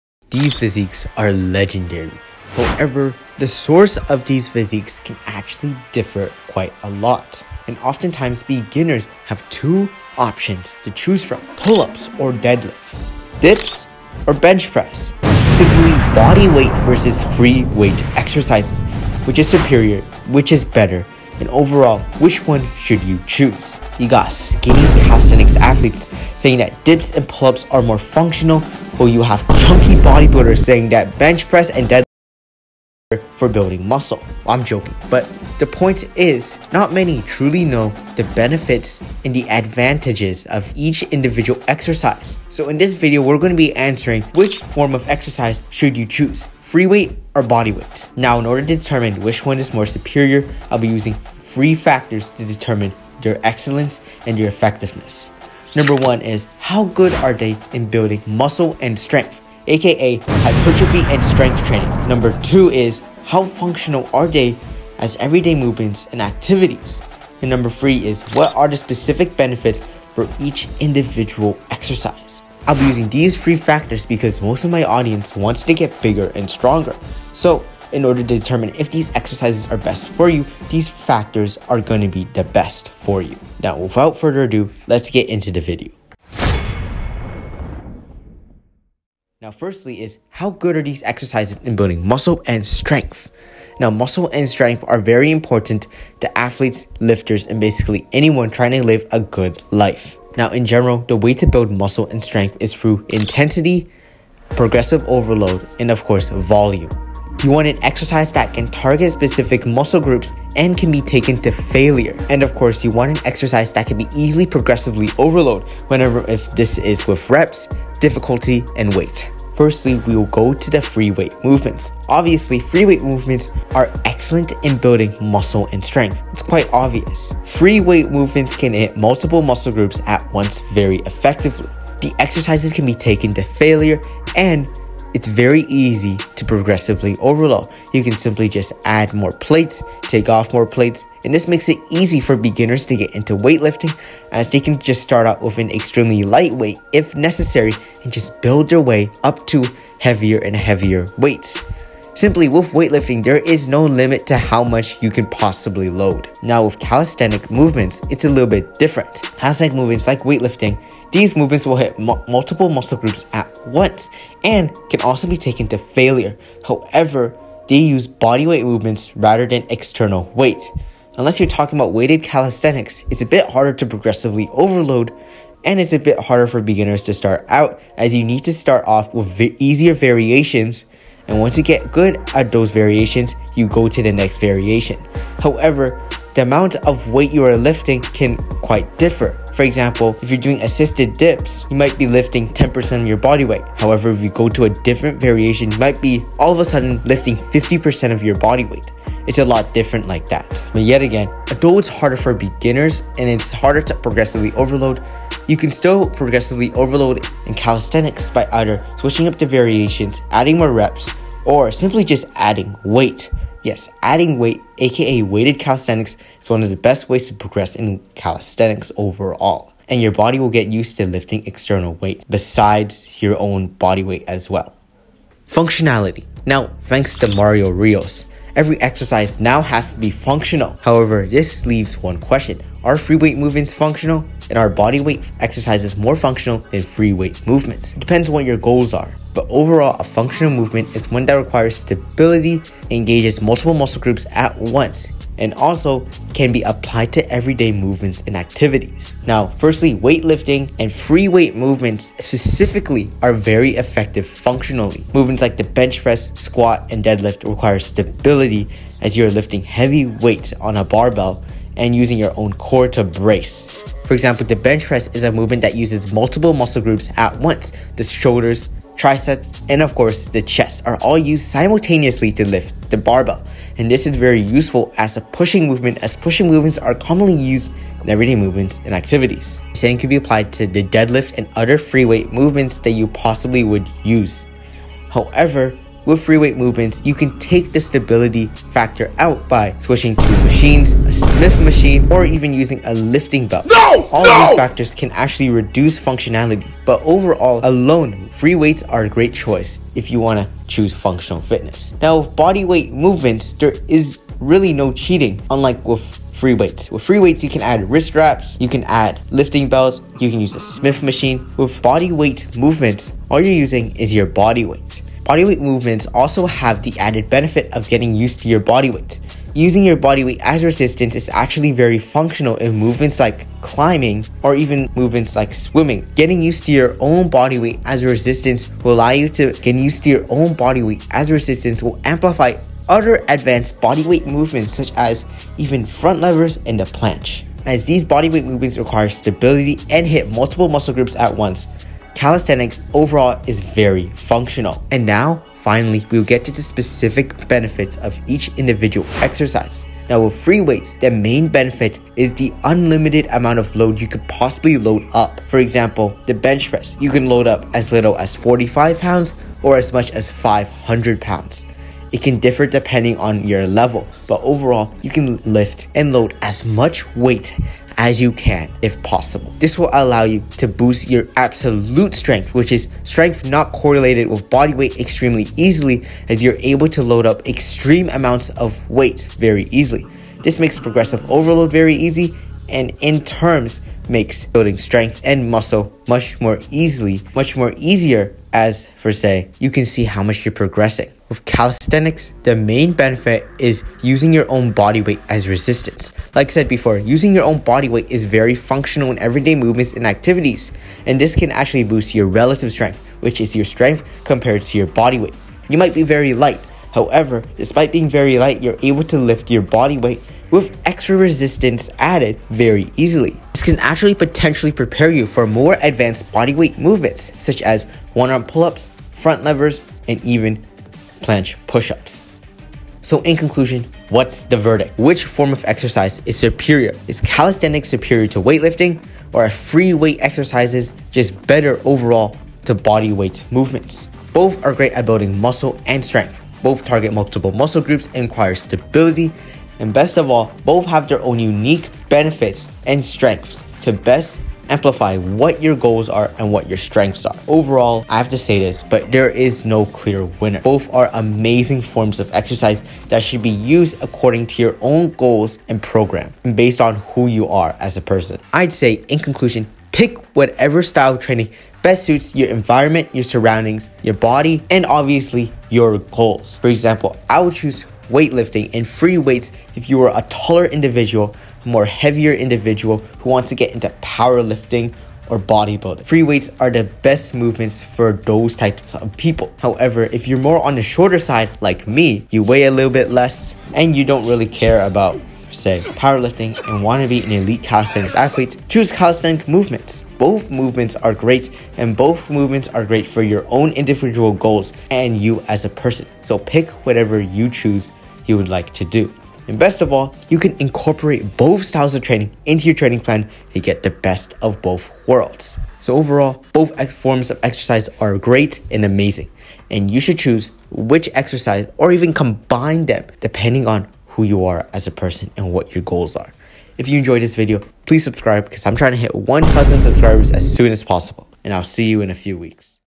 Are Free Weight Movements BETTER Than Bodyweight Exercises?
This video will explain the differences, and benefits and determine which is better between free weight and bodyweight movements. This video will have movements such as the deadlift face-off against pull-ups, and bench press vs weighted dips, which is better and superior.

Time Stamps:
0:00 Introduction
1:28 Building Muscle and Strength
3:44 Functionality
5:44 Specific Benefits of Each Exercise
7:02 The Final Verdict (Which is Better?)

(The first 10 people to join will get a permanent unique role that'll be one to be remembered when I get famous!)